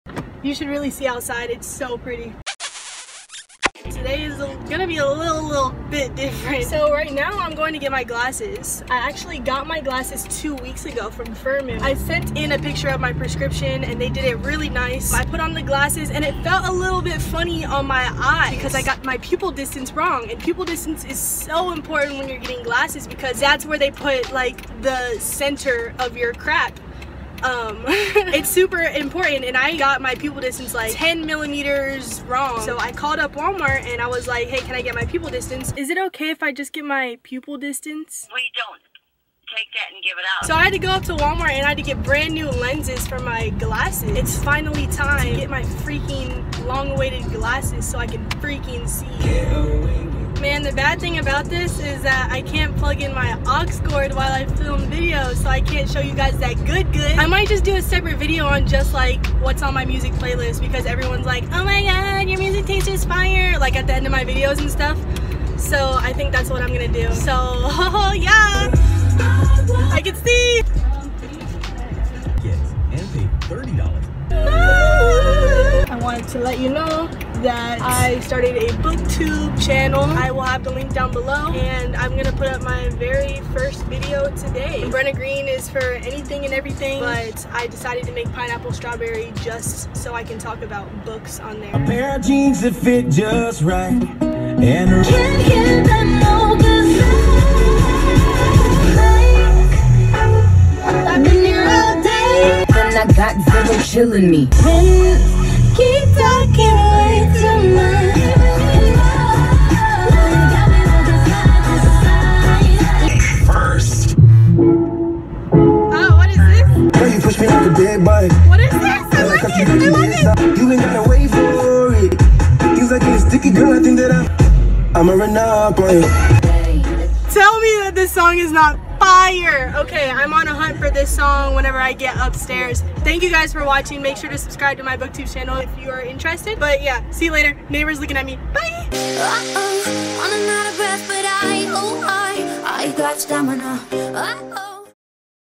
DRIVE WITH ME! 🍍🚗
Drive with me while I go on a magical quest to get glasses :P

23,859• 2,001,462